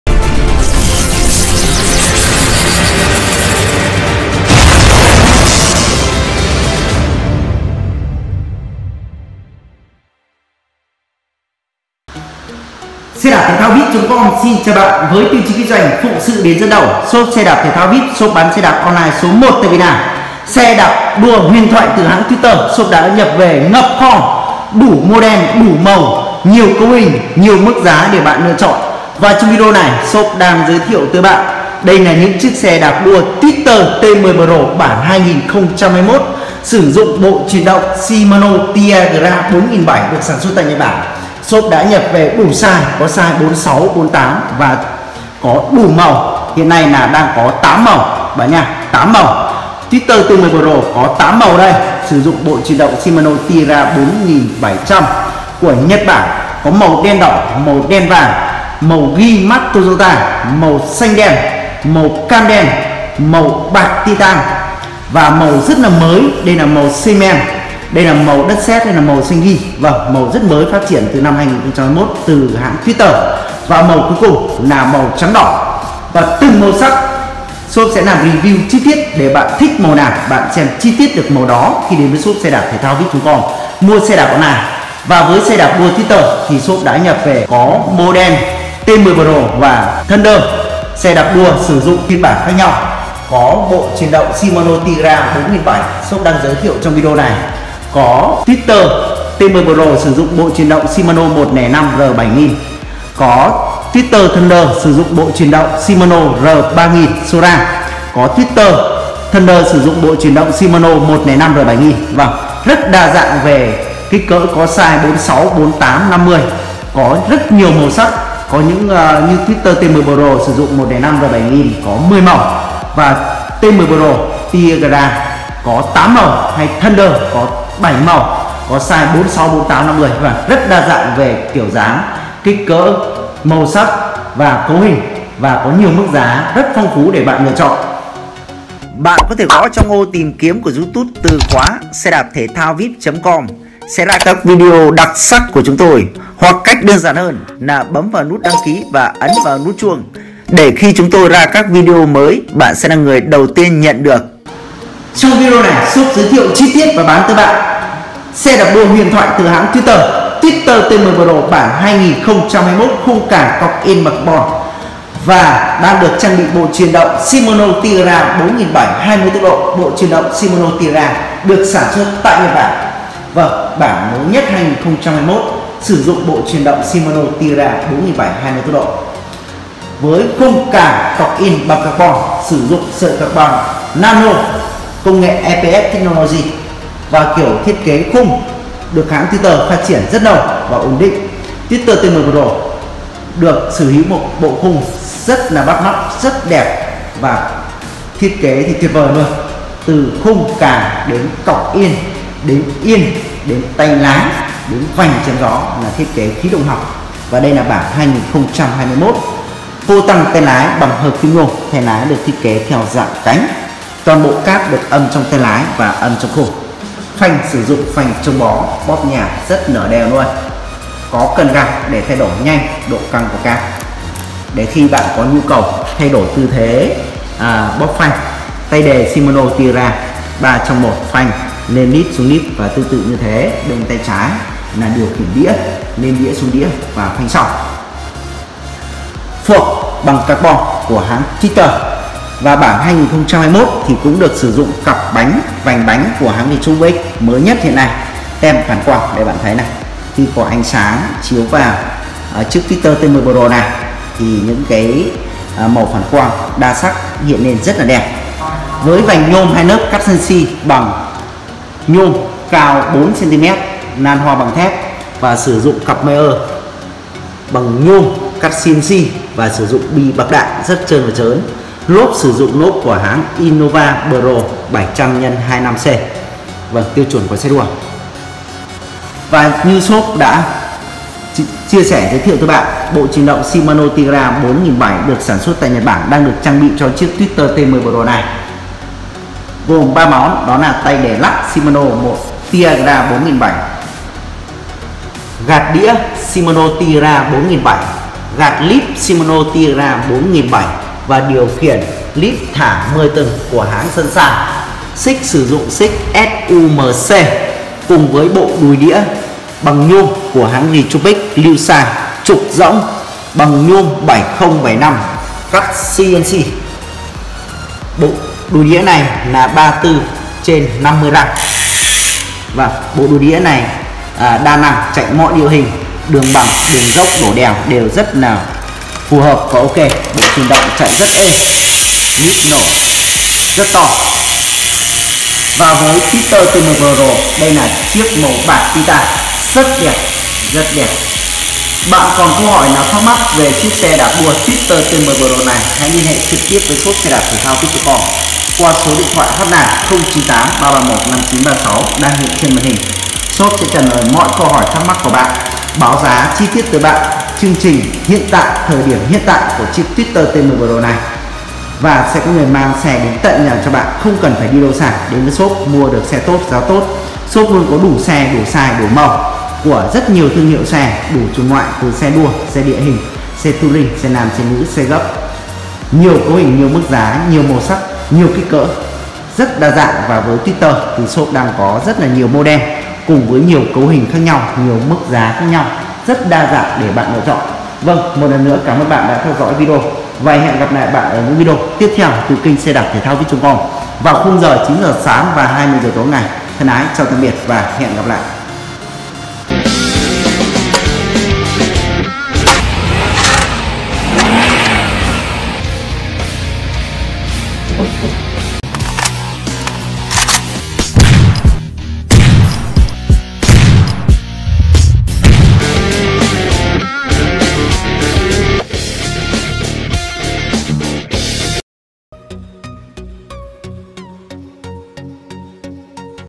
Rẻ 0 lối thoát Xe đạp đua Carbon TWITTER T10 Pro 2021 màu Titan, đồ NHẬT SHIMANO 4700, 0983315936
►Xe Đạp đua Twitter T10 Pro 4700 thiết kế khung+càng +cọc yên carbon Nano và sử dụng bộ truyền động Shimano Tiagra 4700 Japan 2x10 tốc độ của NHẬT BẢN xe vận hành êm ái trơn tru.  Twitter T10 Pro mẫu mã đẹp, bắt mắt hiện đang rất Hiếm và Hot tại thị trường Việt.

►Thông số kỹ thuật xe đạp đua Twitter T10 Pro 4700:
- Size: 46cm - 48cm
- Khung xe : Carbon siêu nhẹ Nano, EPS Technology
- Càng xe : Carbon siêu nhẹ
- Cọc yên : Carbon siêu nhẹ
- Ghi đông/Potang: Hợp kim nhôm Twitter
- Yên xe:  Twitter
- Phanh: C bóp vành Retrospec đời mới
- Tay đề số:  SHIMANO Tiagra 4700 Japan, 2x10 tốc độ
- Tăng tốc trước: SHIMANO Tiagra 4700 Japan
- Tăng tốc sau: SHIMANO Tiagra 4700 Japan
- Đùi đĩa: Trục rỗng Retrospec
- Bàn đạp:  Nhôm CNC
- Xích: 10 speed
- Líp: SUNSHINE 10s- Líp thả
- Vành nhôm Retrospec 2 lớp , cao 4cm
- Mayer: Retrospec bạc đạn
- Lốp: Innova Pro 700x25c